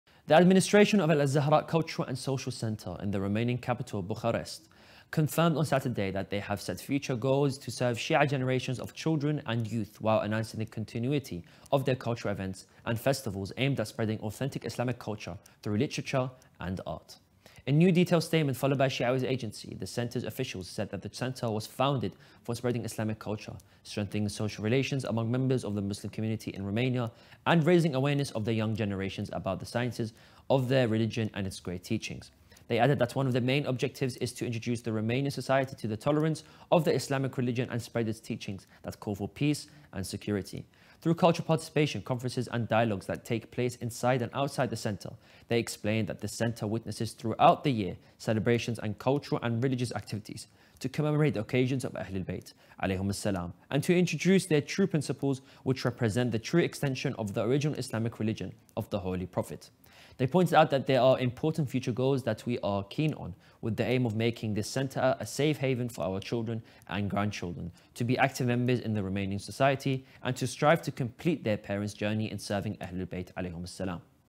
Romania-based Al-Zahraa Cultural and Social Center sets future goals to serve Shia generations
The administration of Al-Zahraa Cultural and Social Center in the Romanian capital, Bucharest, confirmed on Saturday that they have set future goals to serve Shia generations of children and youth, while announcing the continuity of their cultural events and festivals aimed at spreading authentic Islamic culture through literature and art.
In new detailed statement followed by Shia Waves Agency, the center's officials said that "the center was founded for spreading Islamic culture, strengthening social relations among members of the Muslim community in Romania, and raising awareness of the young generations about the sciences of their religion and its great teachings."
They added that "one of the main objectives is to introduce the Romanian society to the tolerance of the Islamic religion and spread its teachings that call for peace and security; through cultural participation, conferences, and dialogues that take place inside and outside the Center."
They explained that "the Center witnesses throughout the year celebrations and cultural and religious activities to commemorate the occasions of Ahlulbayt, peace be upon them, and to introduce their true principles, which represent the true extension of the original Islamic religion of the Holy Prophet."
They pointed out that "there are important future goals that we are keen on, with the aim of making this Center a safe haven for our children and grandchildren; to be active members in the Romanian society and strive to complete their parents' journey in serving Ahlulbayt, peace be upon them."